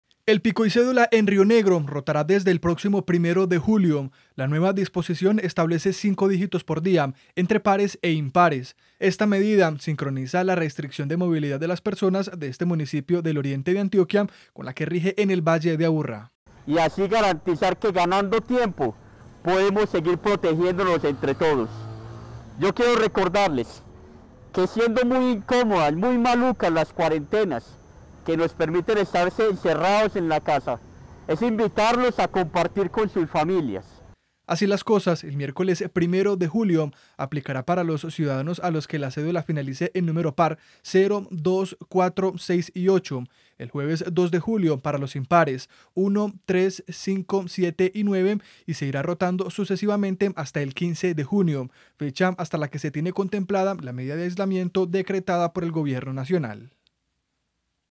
El pico y cédula en Rionegro rotará desde el próximo 1 de julio [Noticias] - Telemedellín
NoticiasTM La nueva disposición establece cinco dígitos por día, entre pares e impares. Esta medida sincroniza la restricción de movilidad de las personas de este municipio del oriente de Antioquia y con la que rige en el Valle de Aburrá.